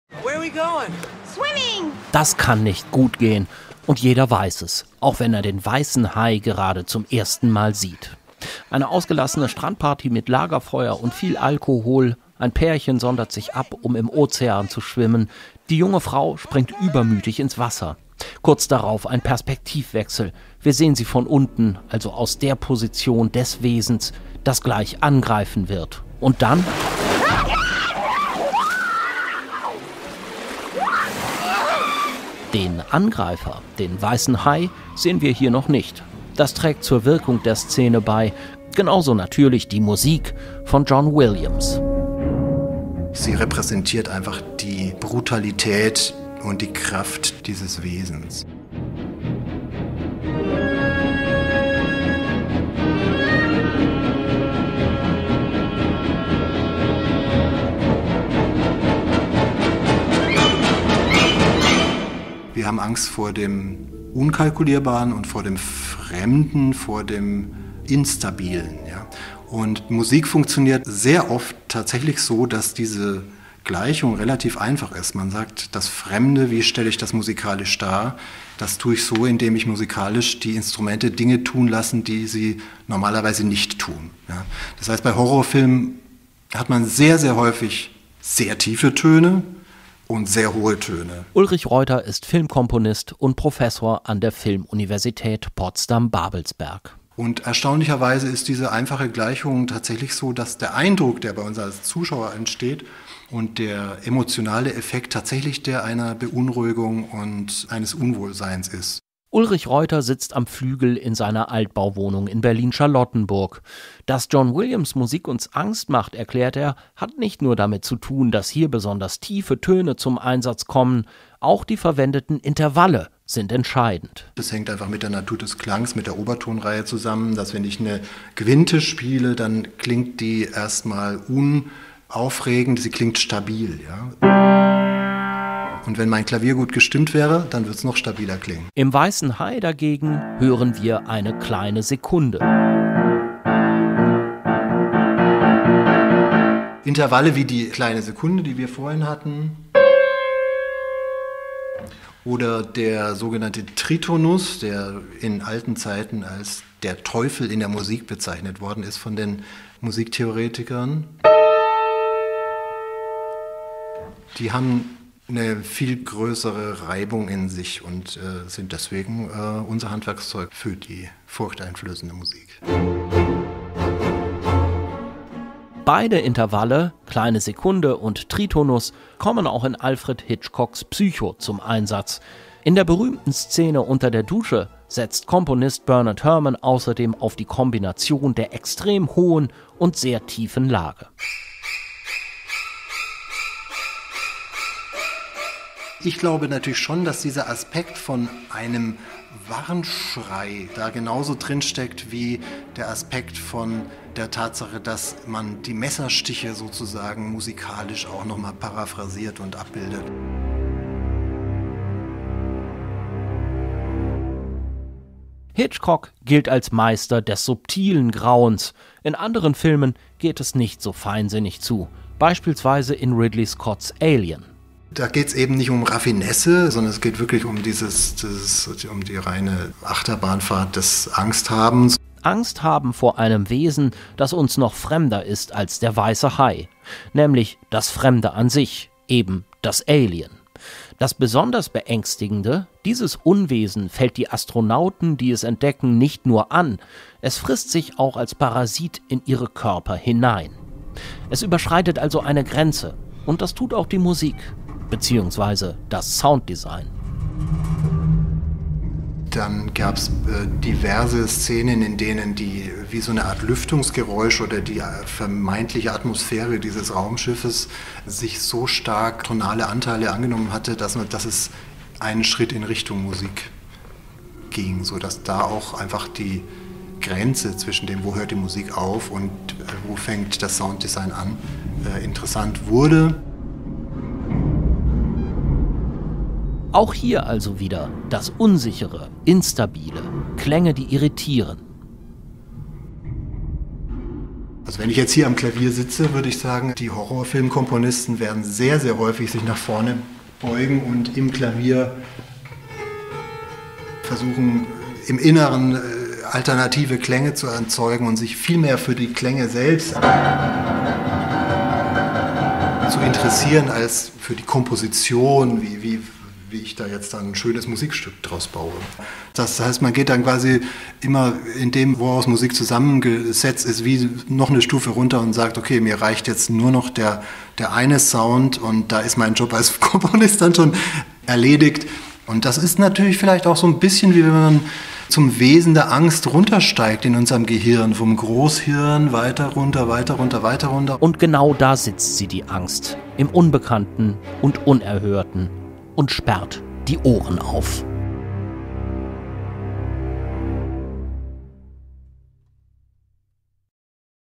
Grauen und Gänsehaut - wie man mit Filmmusik Angst erzeugt
Thriller und Horrorfilme ängstigen nicht nur durch Bilder und Handlung. Auch die Tonspur intensiviert das Grauen. Und zwar oft auf viel subtilere Weise. Der Filmkomponist Ulrich Reuter erklärt wie man mit Tönen Angst erzeugen kann.